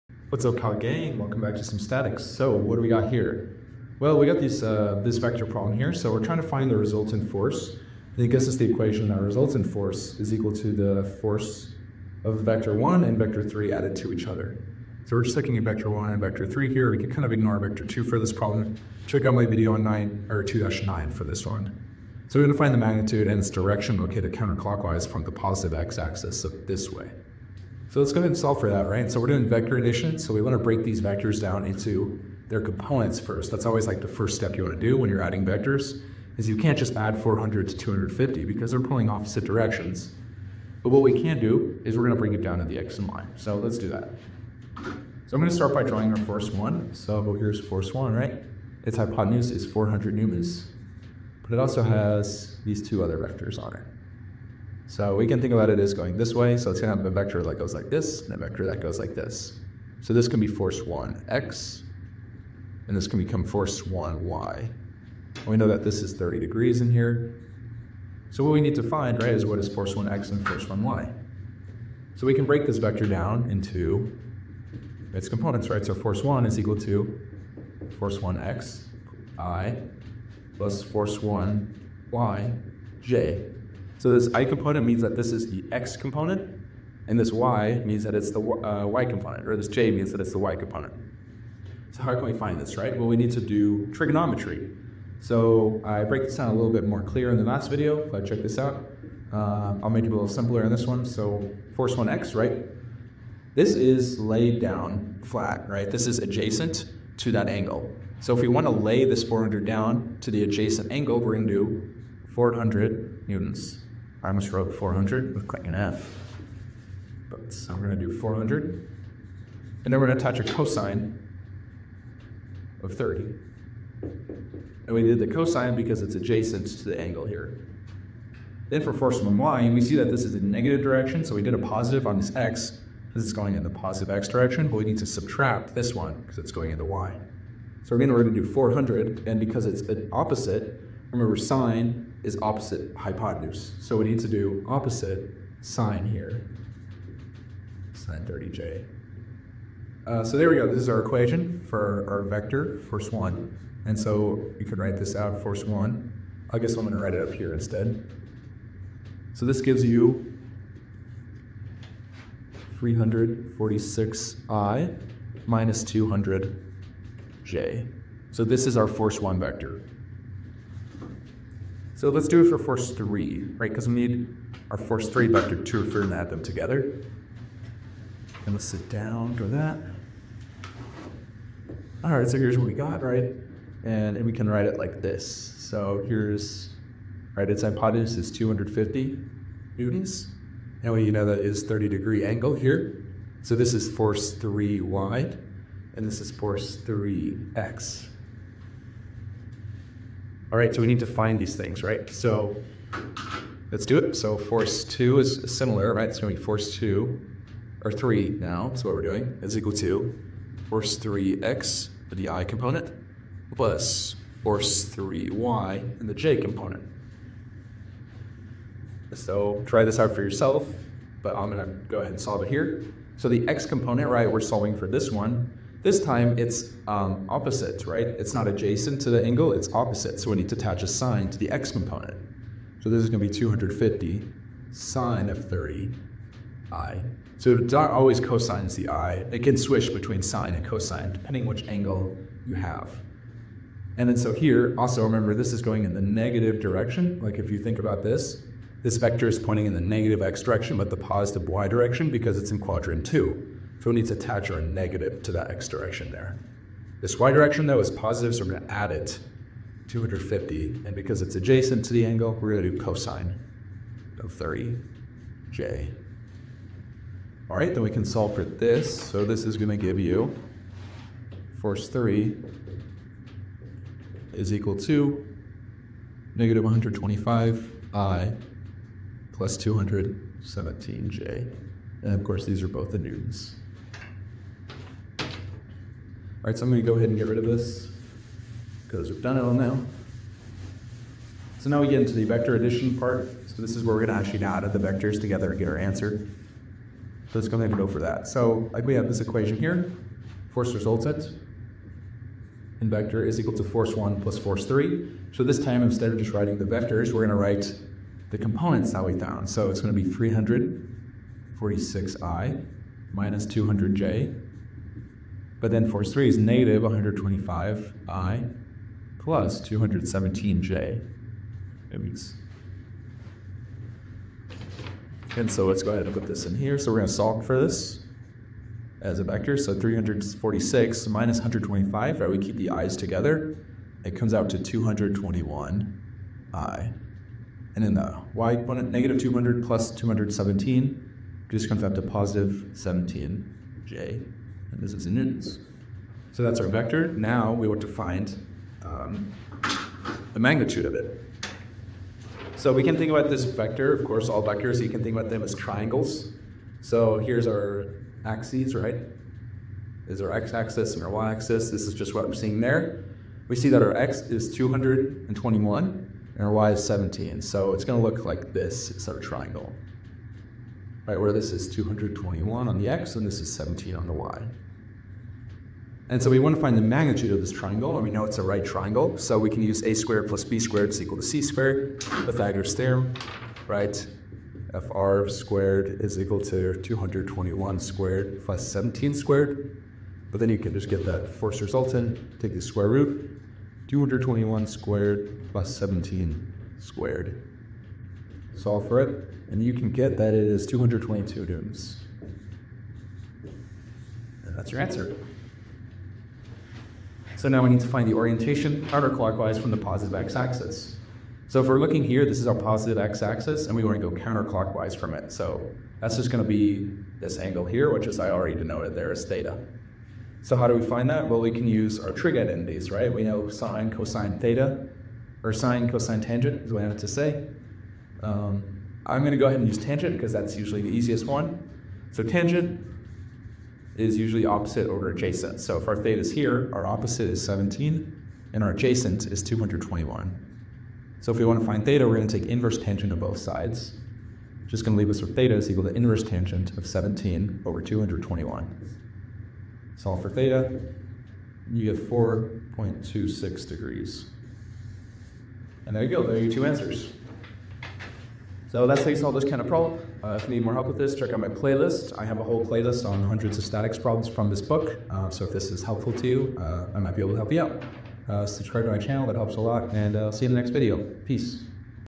Determine the magnitude of the resultant force FR=F1+F3 and its orientation θ - 2-10
2–10. Determine the magnitude of the resultant force FR=F1+F3 and its orientation θ, measured counterclockwise from the positive x axis.